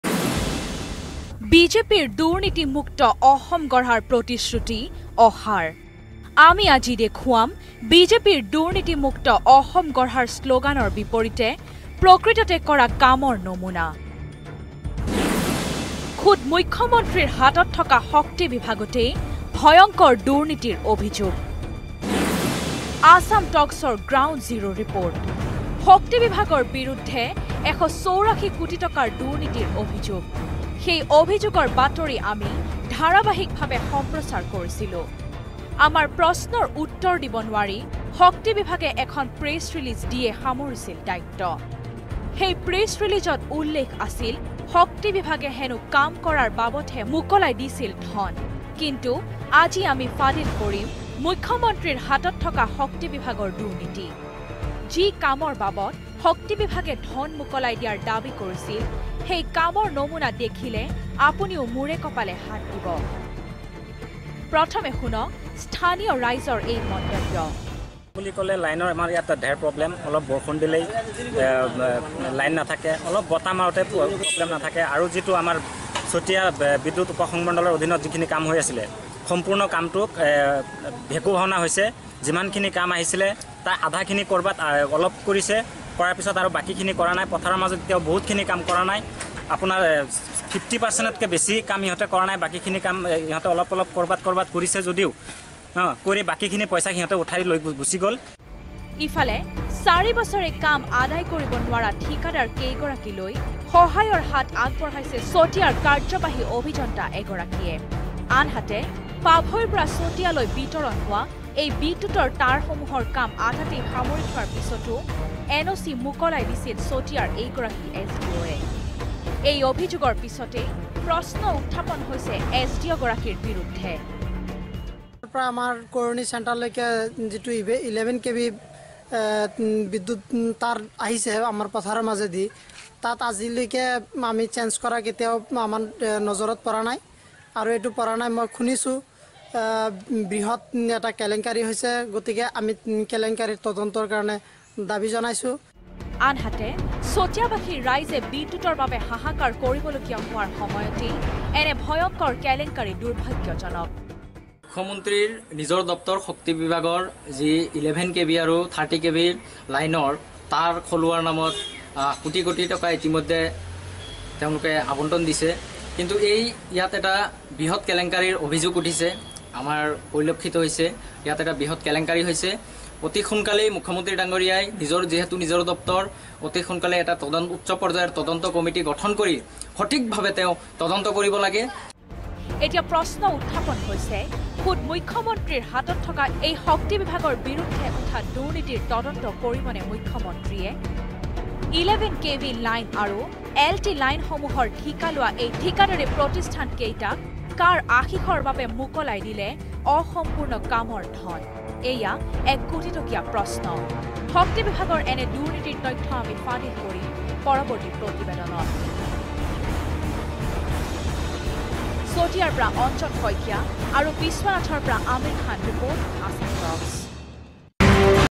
মুখ্য মন্ত্ৰী দায়িত্বত থকা শক্তিবিভাগত ভয়ংকৰ দুৰ্নীতি।
মুখ্য মন্ত্ৰী দায়িত্বত থকা শক্তিবিভাগত ভয়ংকৰ দুৰ্নীতি। ১৮৪ কোটি টকাৰ বিয়াগোম দুৰ্নীতি সংঘটিত হোৱাৰ অভিযোগ পোহৰলৈ আহিছে। কিদৰে সংঘটিত হৈছে এই দুৰ্নীতি চাওঁ আহক আন এক প্ৰতিবেদন।